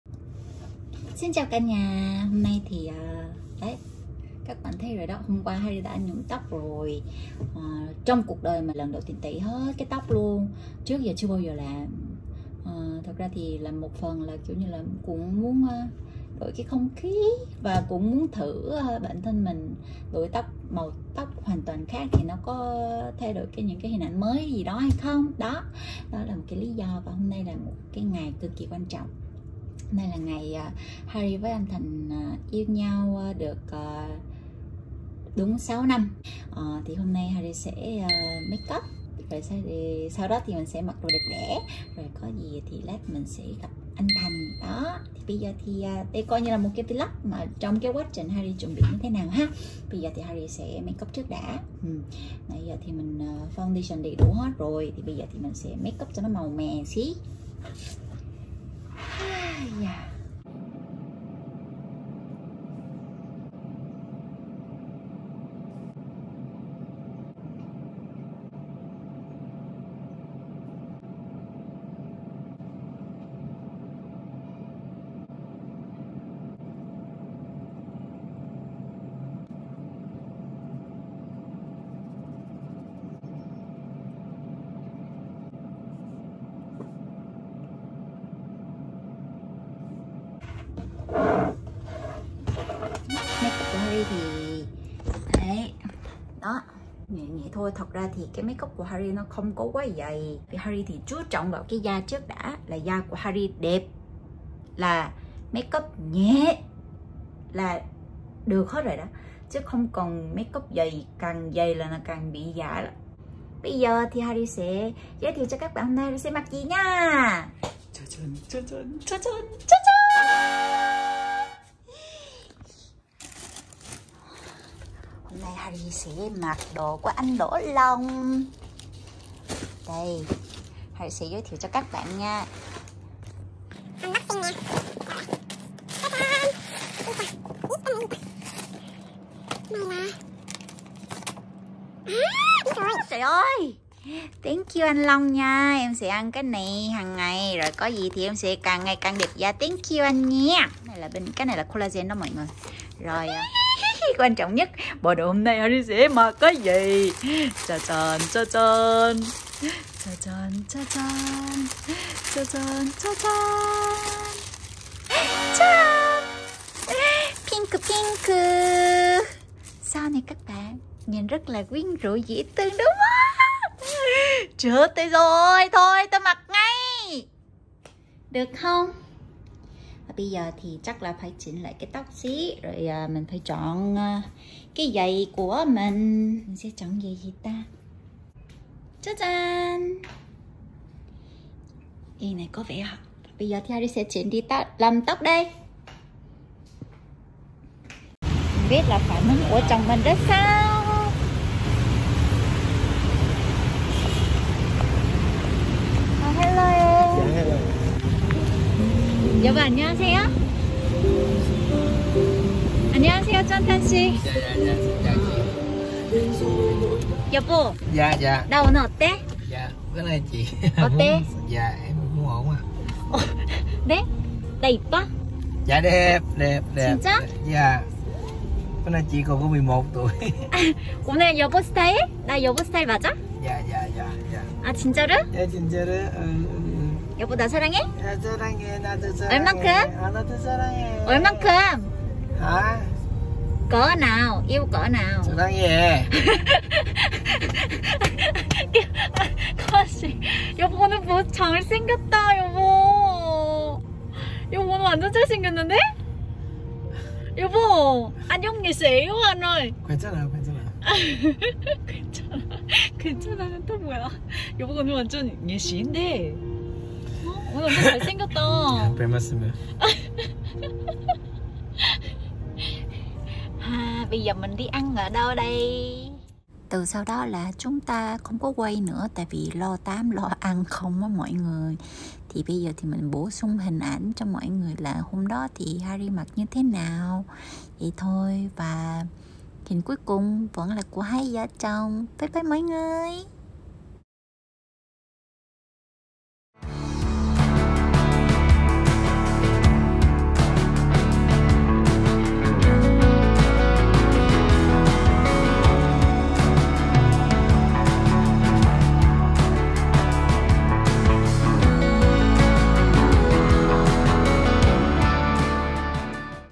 Hari "lên đồ" đi hẹn hò với A Xìn
Hari Won "lên đồ" đi hẹn hò với Trấn Thành

목욕을 안하면 자꾸 떡이 지는 깜시
오랜만에 쩐탄이 목욕을 시키는데...

♥ 구독해서 하리원의 최신 뮤직비디오를 확인하세요!

많이 많이 사랑해주세요